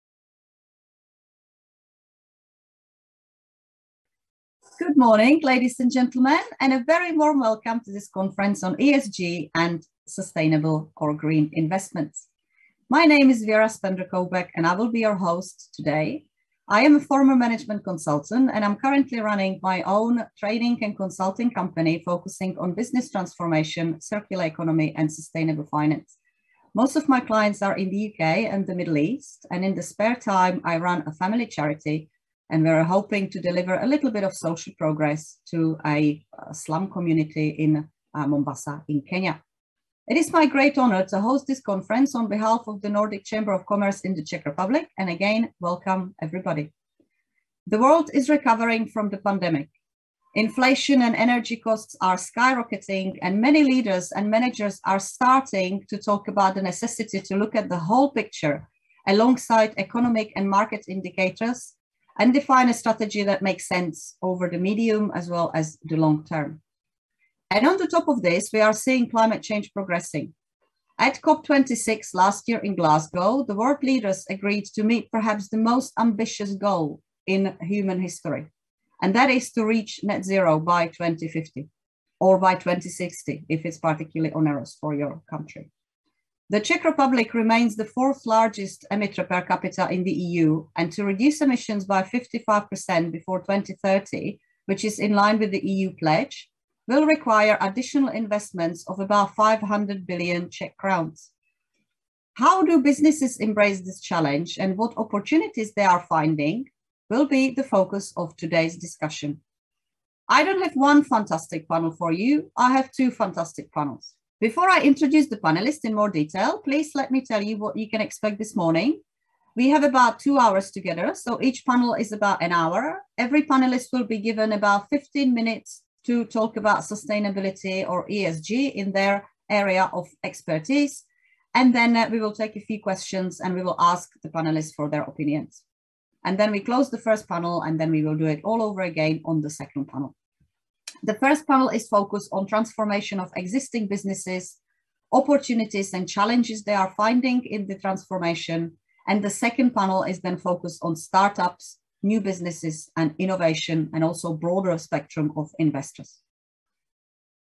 ESG conference introduction by Vera Spender Koubek, Moderator
In this workshop, we will discuss how the financial sector is responding to this goal and how it embraces the broader ESG challenges. A key role of the financial sector is to support individuals, SMEs, large companies as well as policy makers, which makes it arguably the most important sector when it comes to net zero and sustainability.

We will focus on the current ESG trends as well as future plans and we will discuss how creating sustainable corporate action can mitigate risks and brings social, environmental as well as economic benefits